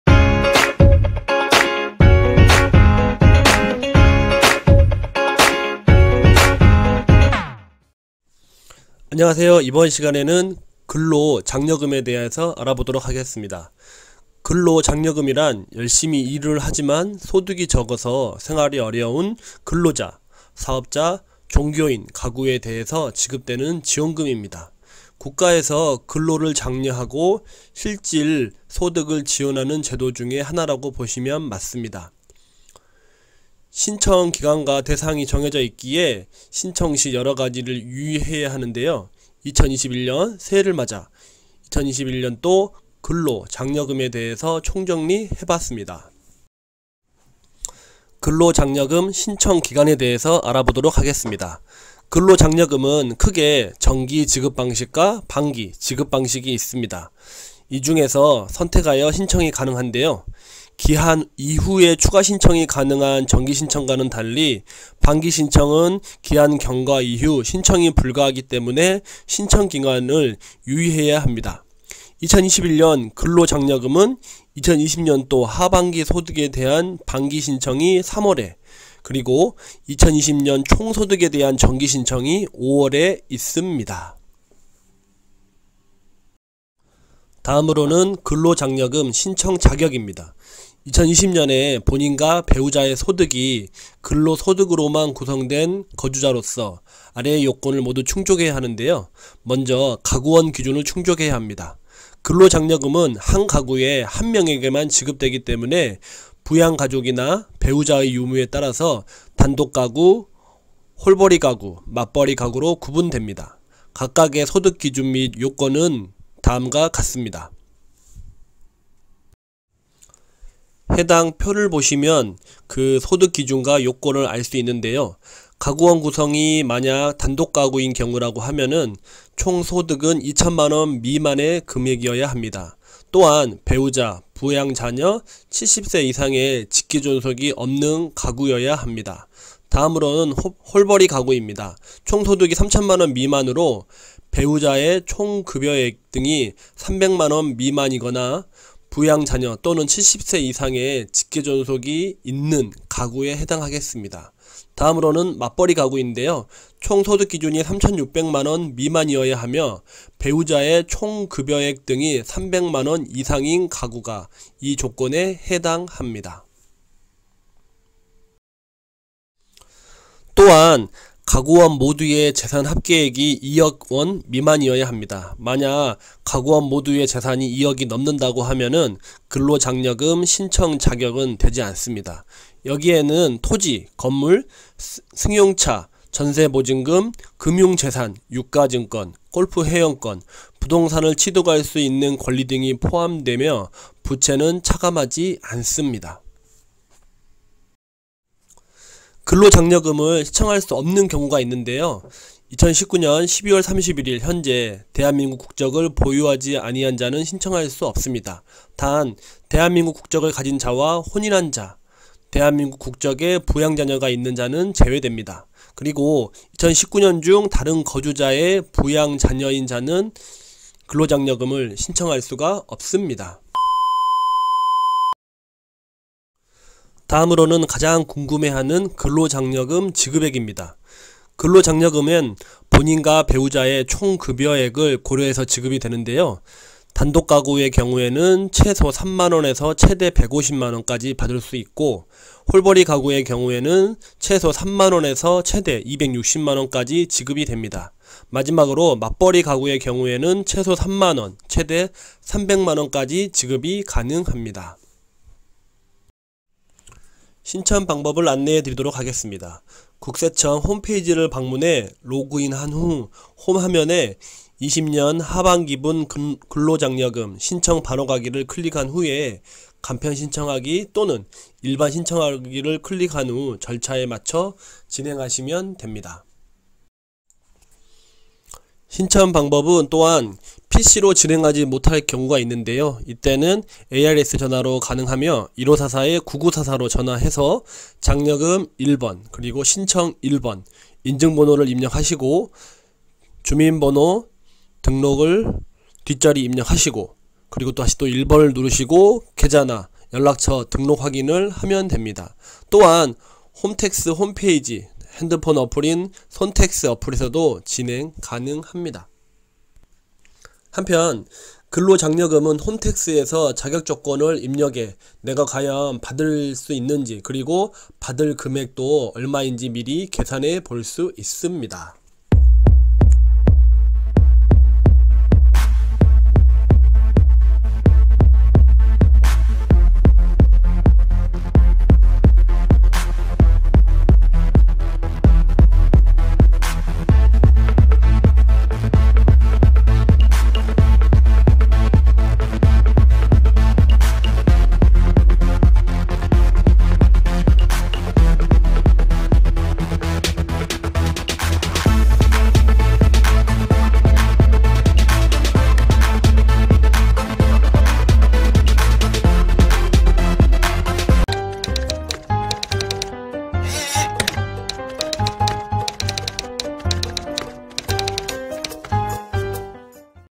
근로장려금 신청, 자격요건, 지급일 최대 지급액은? 총정리
■근로장려금이란?

근로자, 사업자, 종교인 가구에 대해 지급되는 지원금이다.
국가에서 근로를 장려하고 실질소득을 지원하는 제도 중의 하나이다.

신청 기간과 대상이 정해져 있기에 신청시 여러 가지를 유의해야 한다.
근로장려금에 대해 총정리 해봤습니다.

근로장려금 1원이라도 받았다면 당장 혜택 신청

국세청 홈페이지를 방문해 로그인한 후 홈 화면에 '20년 하반기분 근로장려금 - 신청 바로가기' 클릭 후 간편신청하기 또는 일반신청하기를 클릭한 후 절차에 맞춰 진행하면 된다.

PC로 진행하지 못한 경우 ARS전화로 가능하며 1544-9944로 전화해 장려금 1번-신청 1번-인증번호 입력-주빈등록번호 뒷자리 입력-1번-계좌, 연락처 등록확인을 하면 된다.

또한 홈택스 홈페이지, 핸드폰 어플인 손택스 어플에서도 진행 가능하다.

한편 근로장려금은 홈텍스에 자격조건을 입력해 내가 받을 금액을 미리 계산해볼 수 있다.

0:00 도입부
0:08 근로장려금이란?
0:47 근로장려금 신청기간
3:18 근로장려금을 신청할 수 없는 경우
3:46 근로장려금 지급액